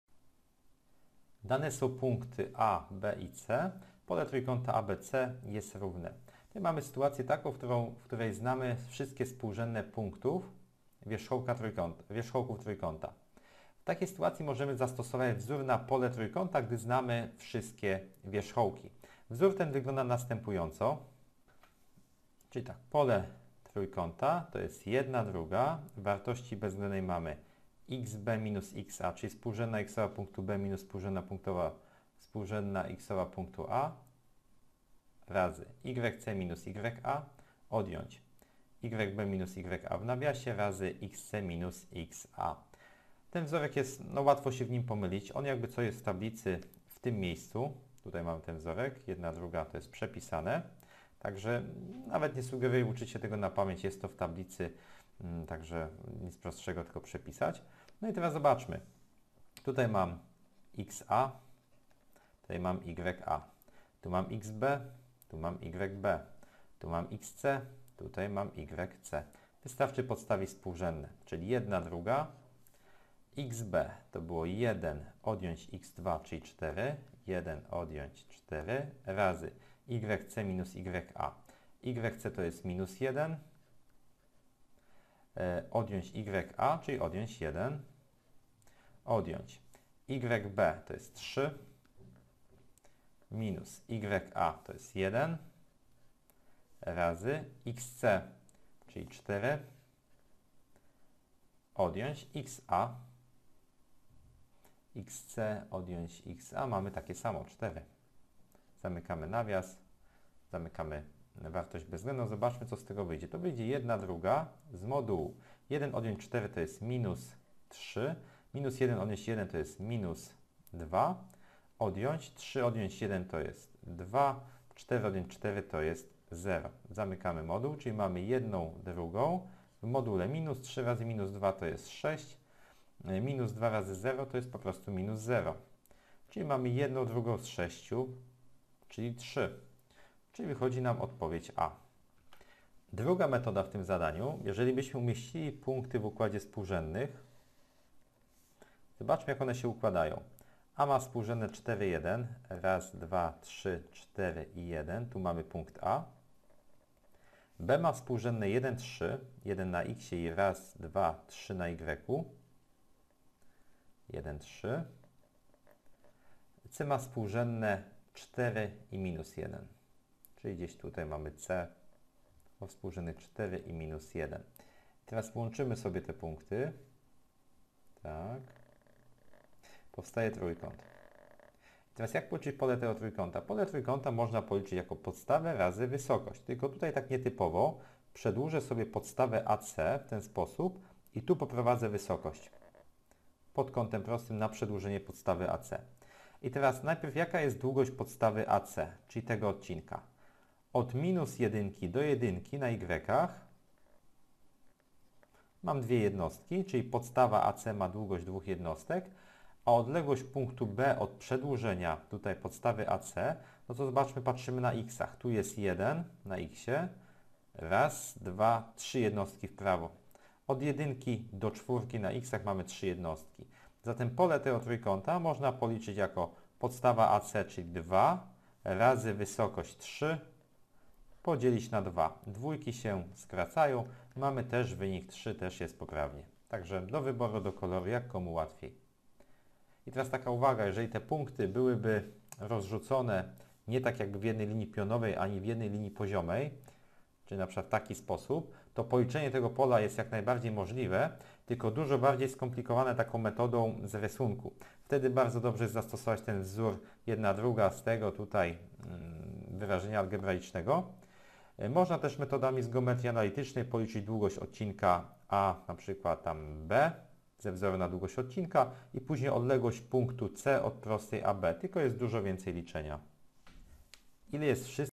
22 Dane są punkty A=4,1, B=1,3, C=4,−1 Pole trójkąta ABC jest równe
Dane są punkty A=(4,1), B=(1,3), C=(4,−1). Pole trójkąta ABC jest równe Źródło